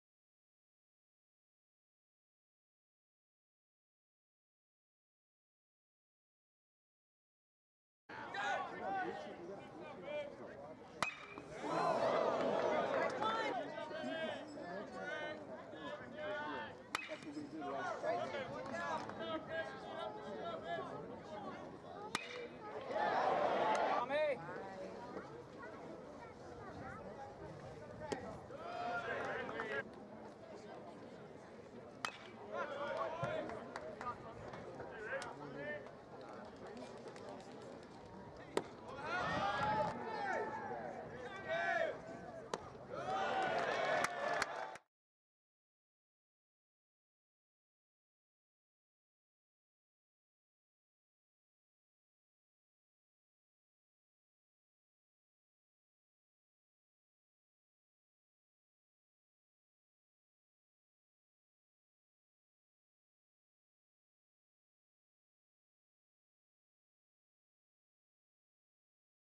Spencer Brickhouse (3-30-2018) vs Wichita State Univ.
Spencer Brickhouse (1B/DH, East Carolina Univ.) vs Wichita State Univ. (Greenville, NC).